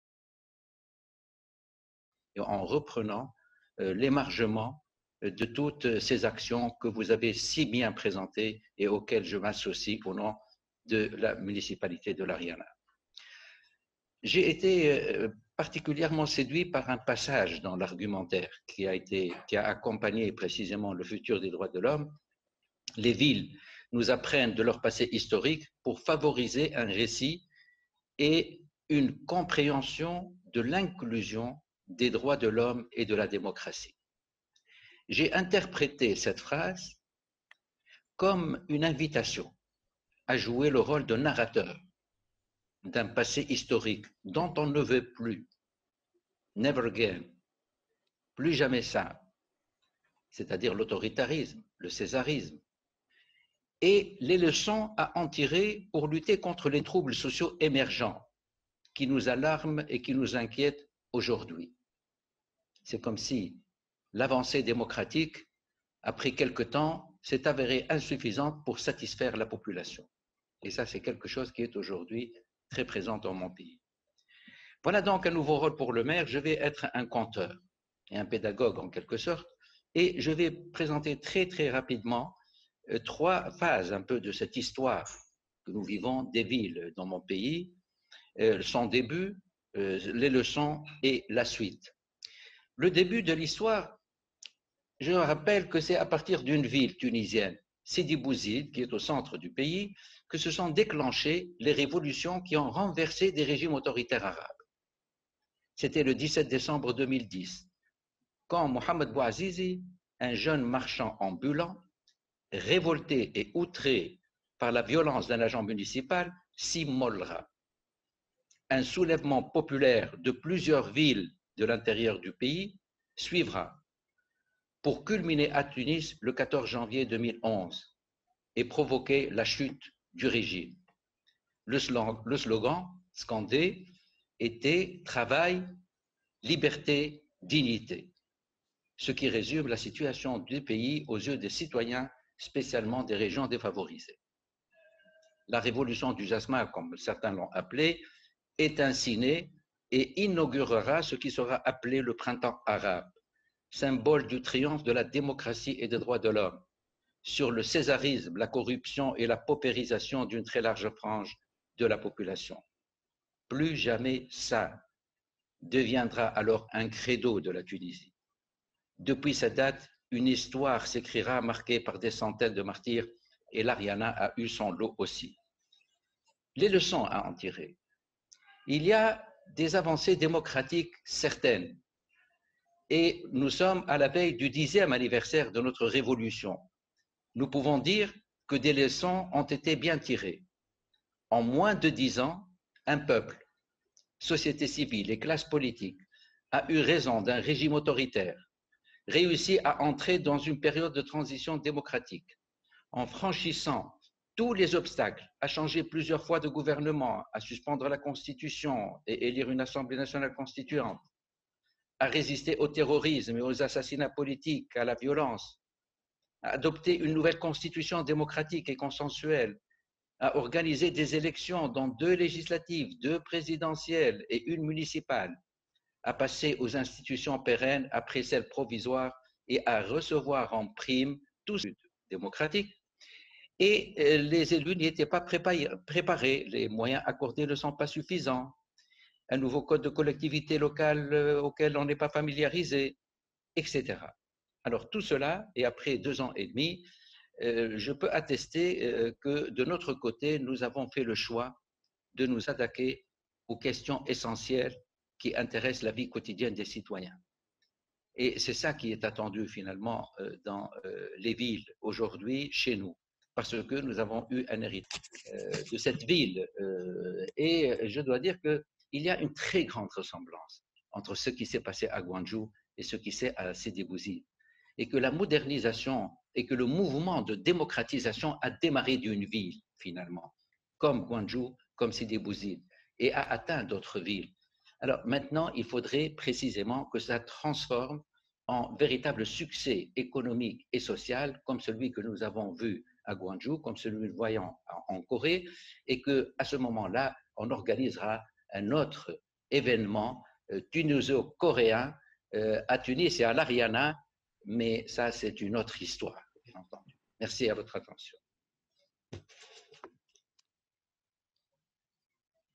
[The10thWHRCF] Mayor of Municipality of Ariana, Fadhel Moussa's Speech (Eng Sub)
[Plenary Session 1] Speech of Fadhel Moussa, Mayor of Ariana, Tunisia
Title: Addressing Social Unrest and Learning from the Historical Past

출처: 세계인권도시포럼 사무국, 아리랑 TV
Credits: World Human Rights Cities Forum Secretariat,  Arirang TV